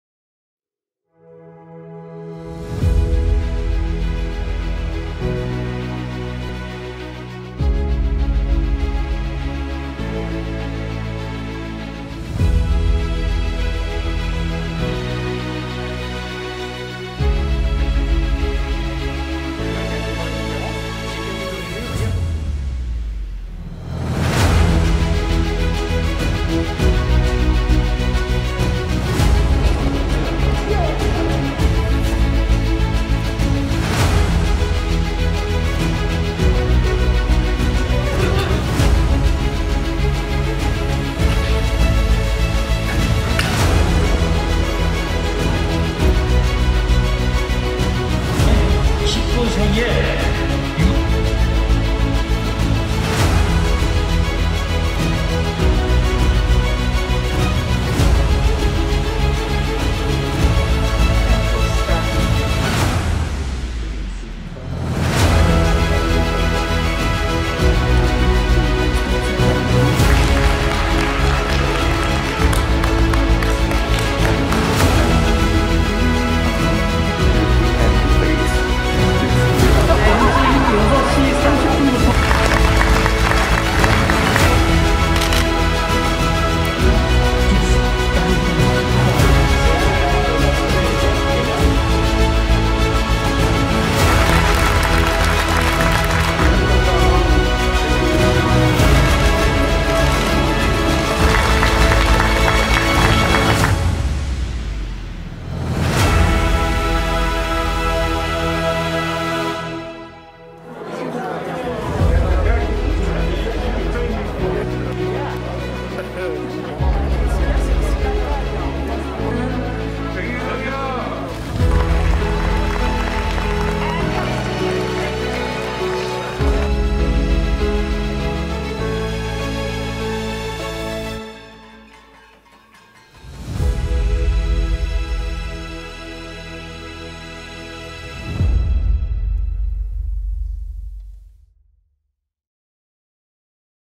Highlights from the World Cooperative Congress
Themed "Deepening the Cooperative Identity", the World Cooperative Congress took place on 1-3 December in Seoul, the Republic of Korea. Over 1,700 delegates from around the world participated in-person or online.